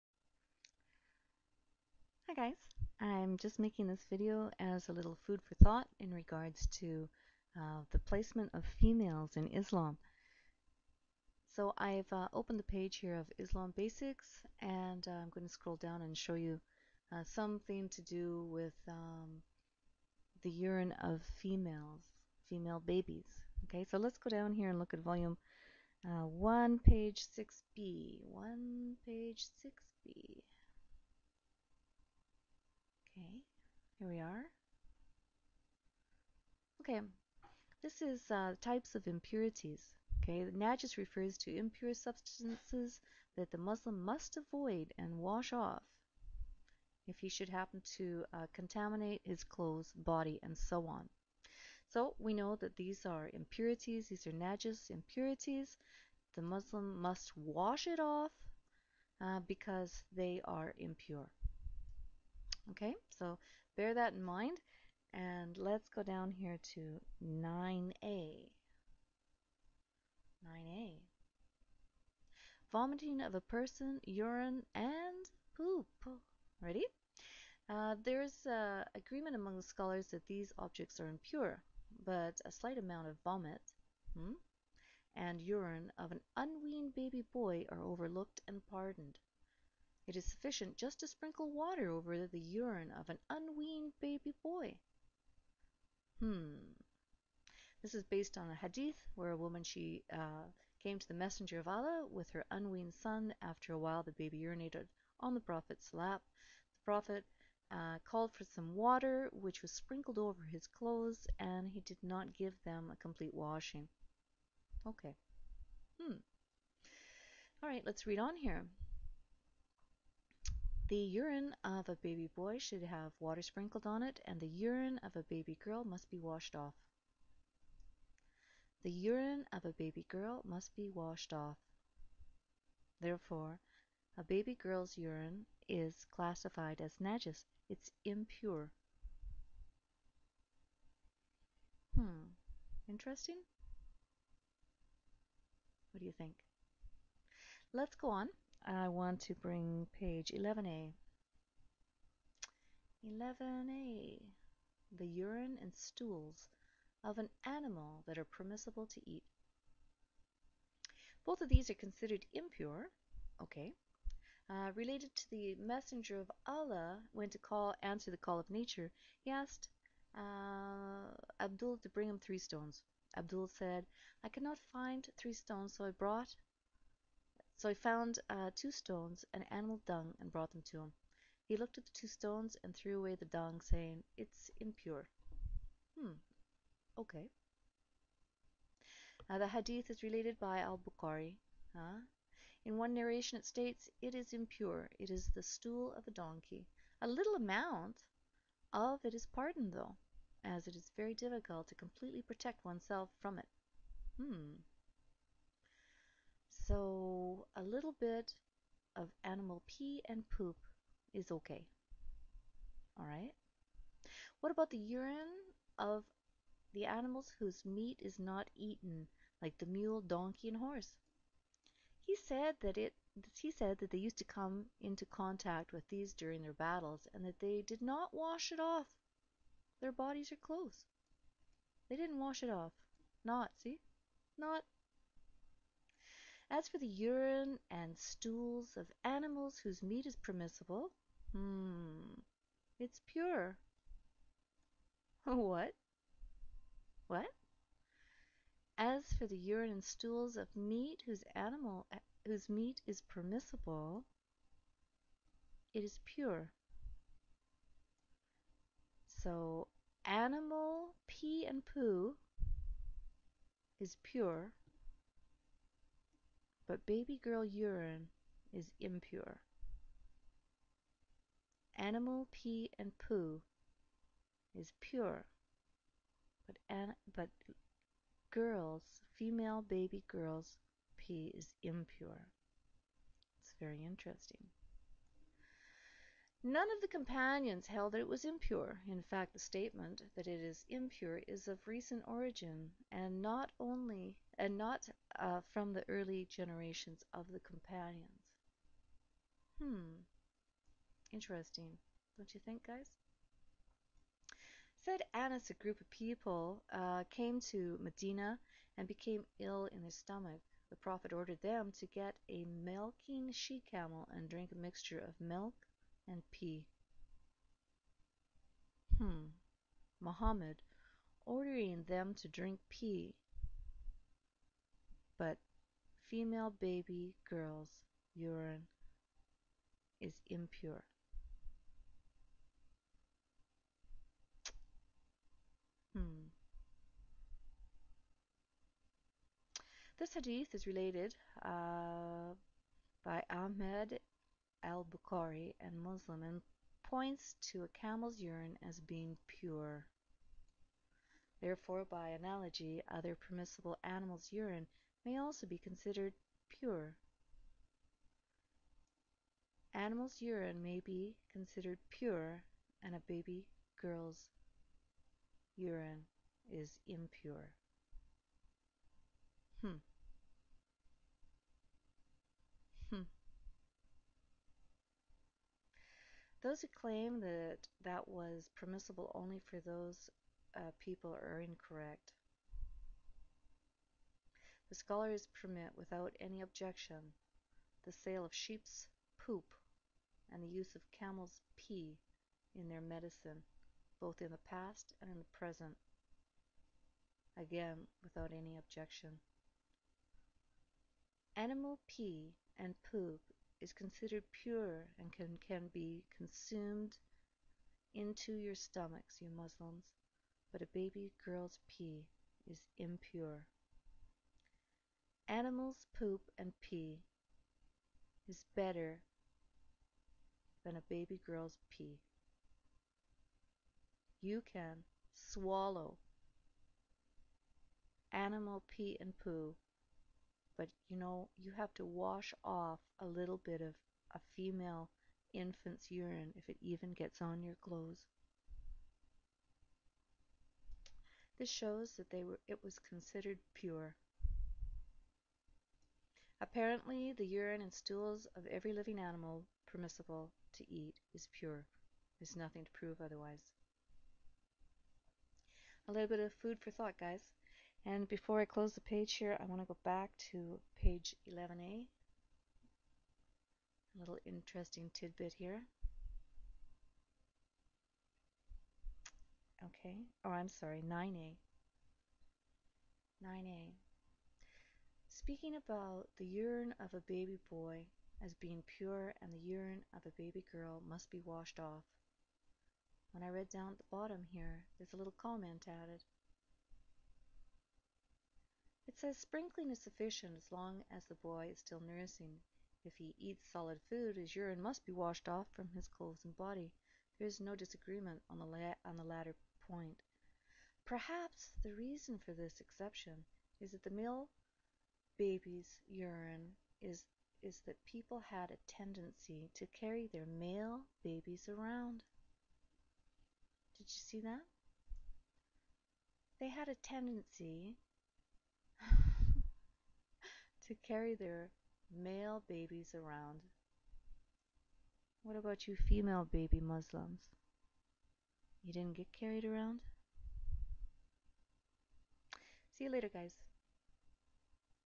Islam's teachings on piss and poop
According to Mohammed, the animal piss/poop is clean.  That's good to know Mohammed.